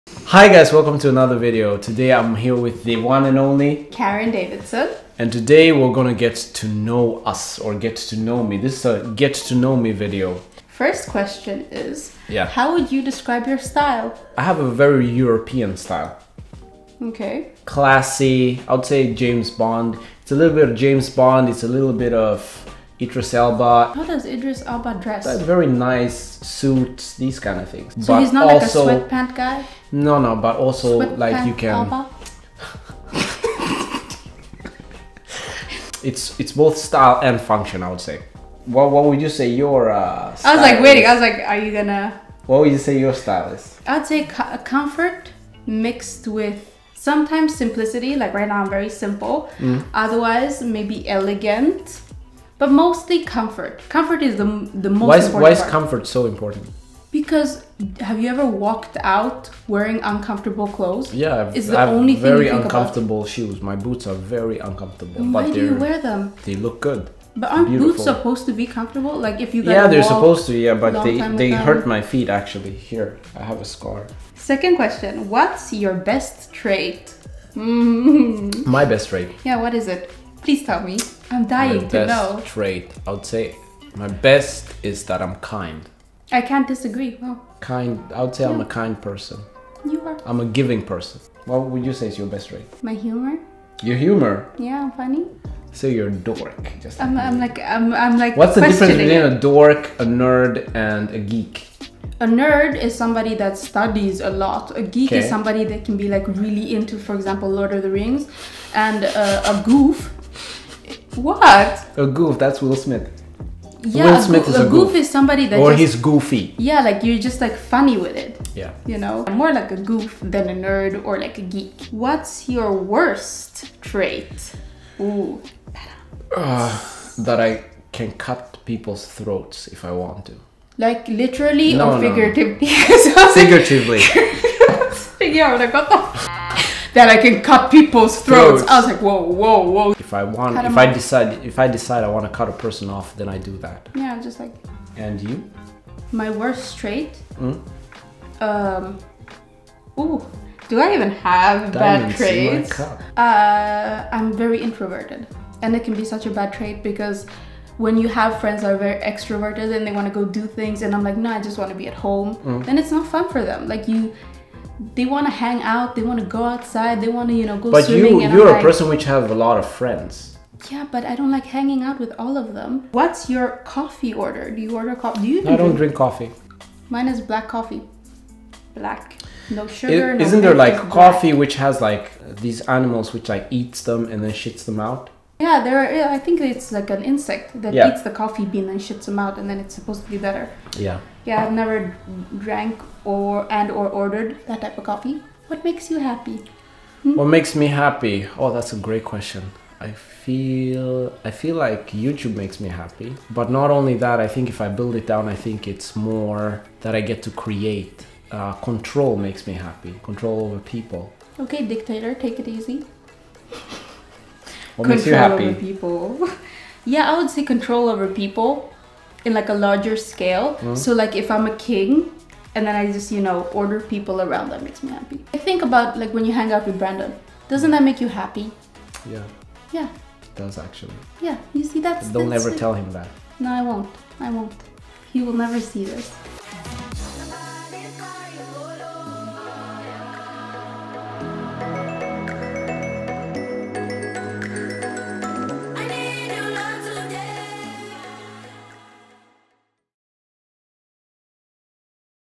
Iyambo and Karin - Get to know me video. Brother and Sister from Namibia and Sweden
In this youtube video Brother and sister Moses Giovanni and Karin Davidsson, talk about themselves and give you a look into how they are. They discuss personality traits and what they have in common.
Watch like and explore this channel.